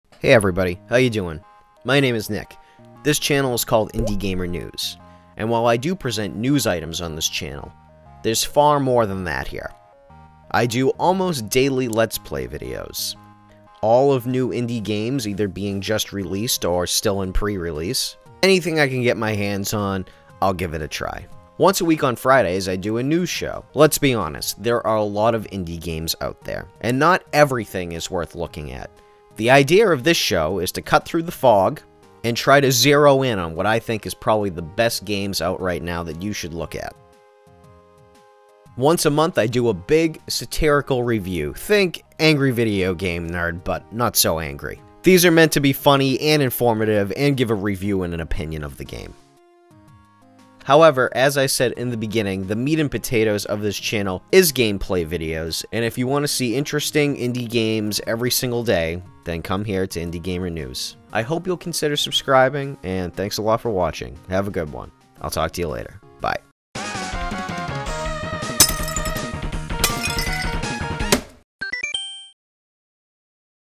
Indie Gamer News - Channel Trailer - WHAT DO I DO HERE!?
Hey!

Here you will find Indie Game let's plays, News updates and Reviews! Plus I'm a sarcastic jerk! You'll love it!

Hi! I'm Nick. I sound like a big faceless corporation, but I'm just a big faceless guy!